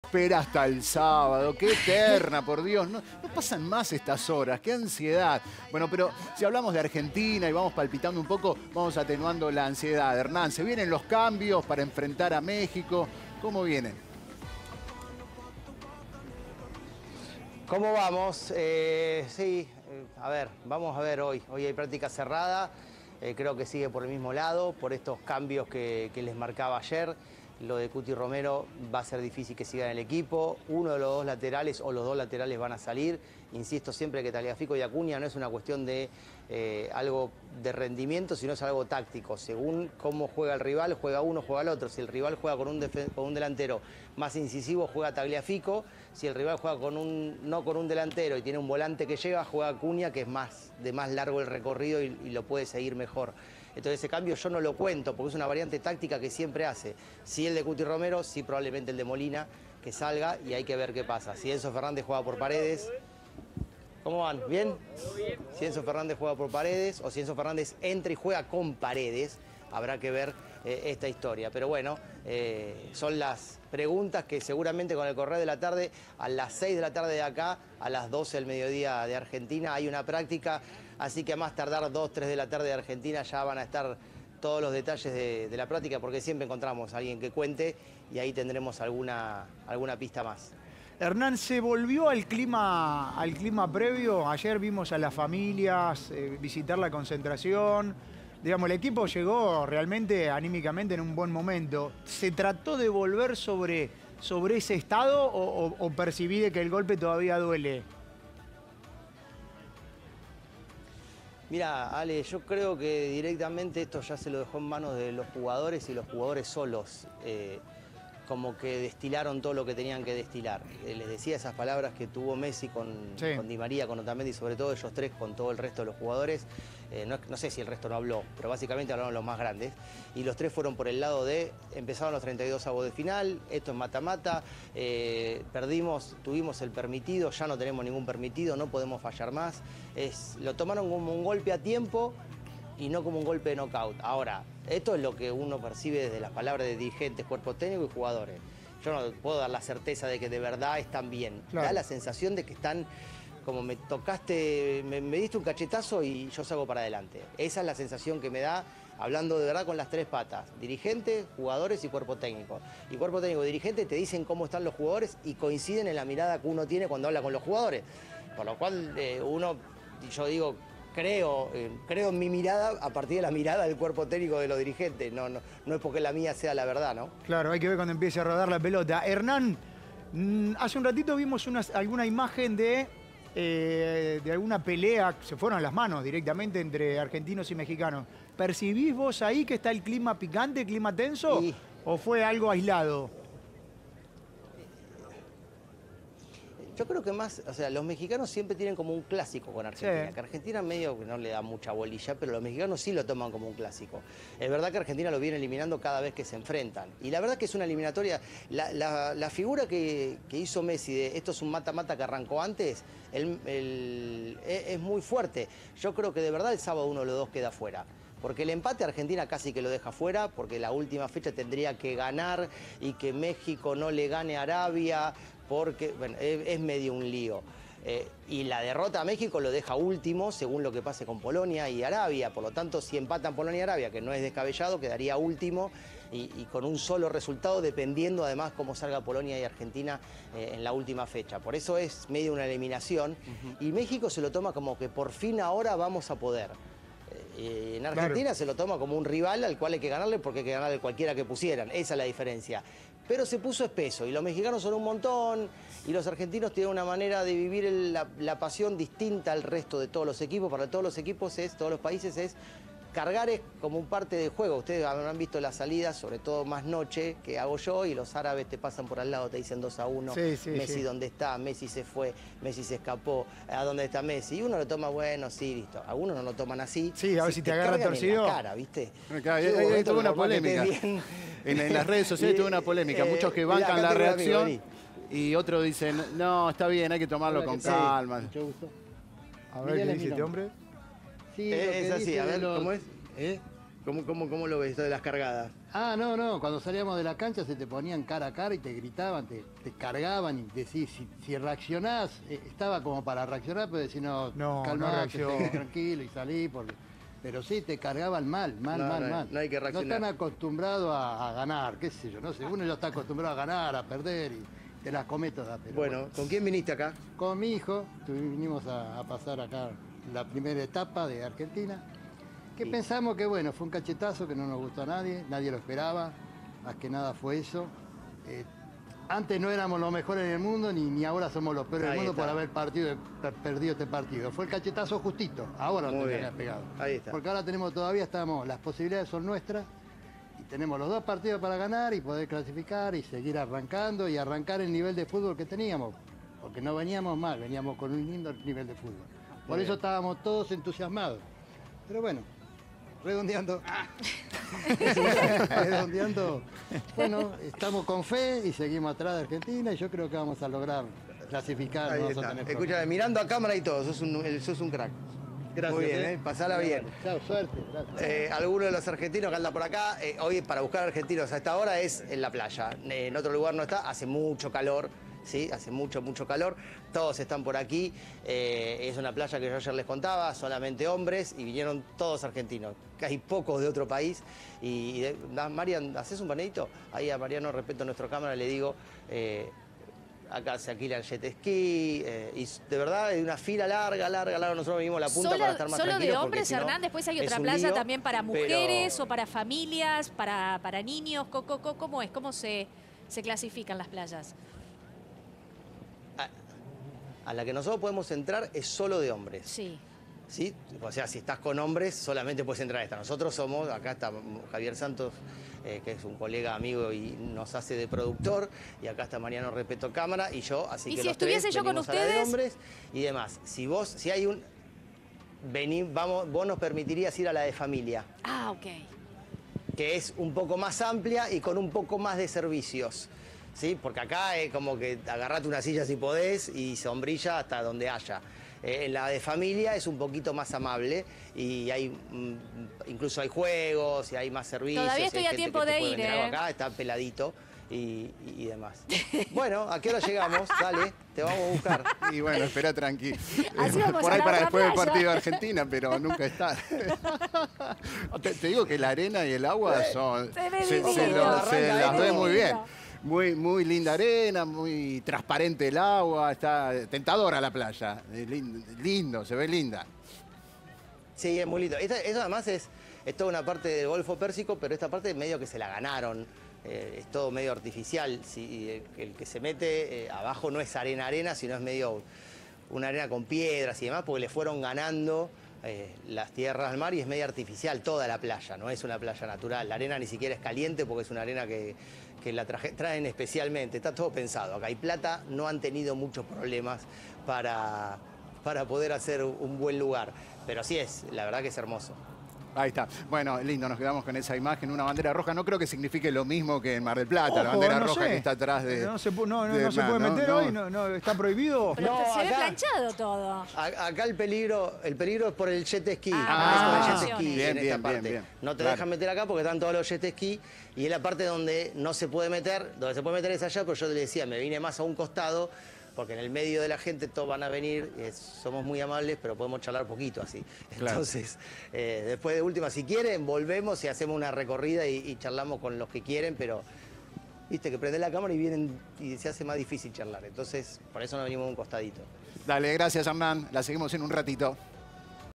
La Selección Argentina entrena para el sábado: cómo se sospecha que será la formación | 9 de Área
Todos los detalles de La Scaloneta desde Qatar por Hernán Castillo.